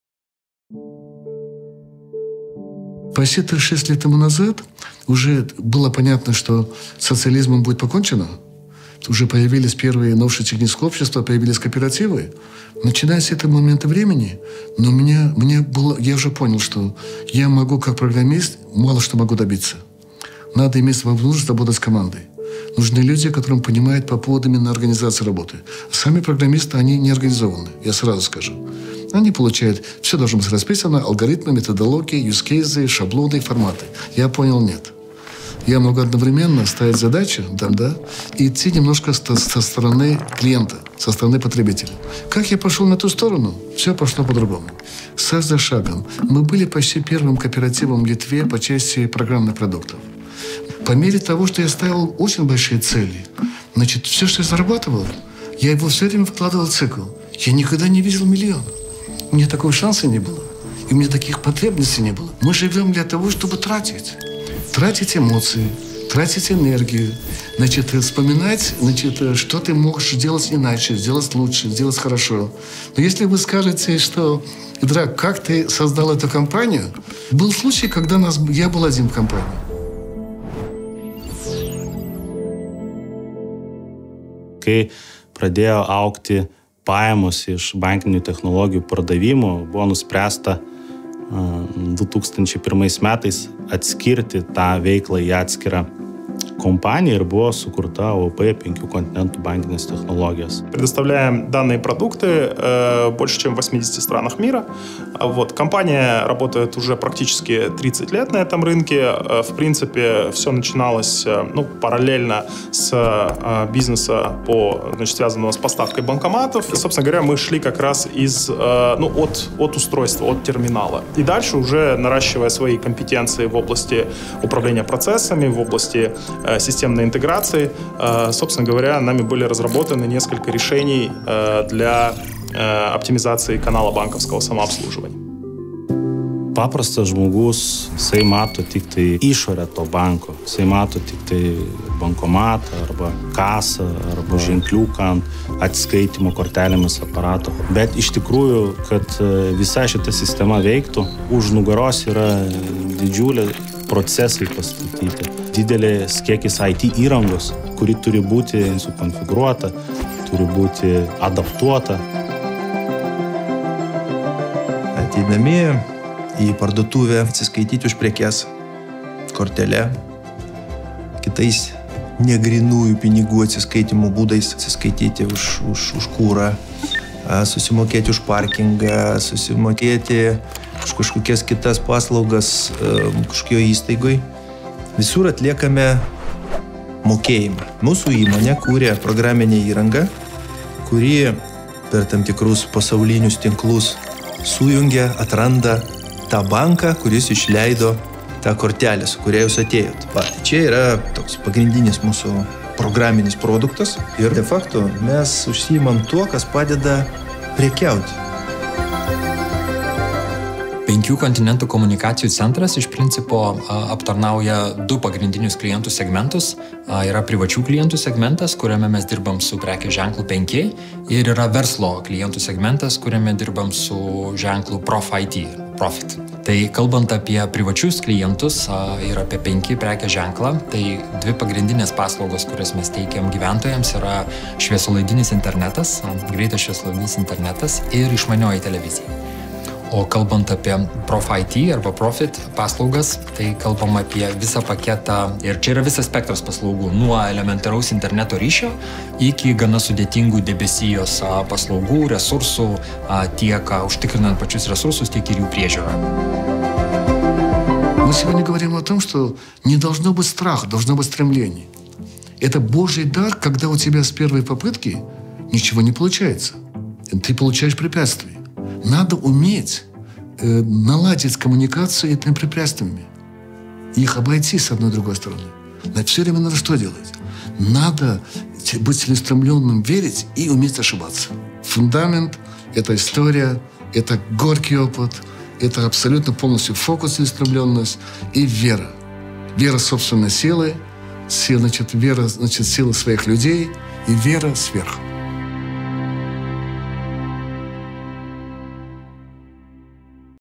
Penki Kontinentai. We create history
The Penki Kontinentai group has been an information technology developer and a pioneer of many innovations in Lithuania for thirty years, and has also successfully established itself in foreign markets. Film “Five Continents. We Create History ”tells about what we believe in, what has guided us all these years and how the history of the group of companies is reflected in our work today.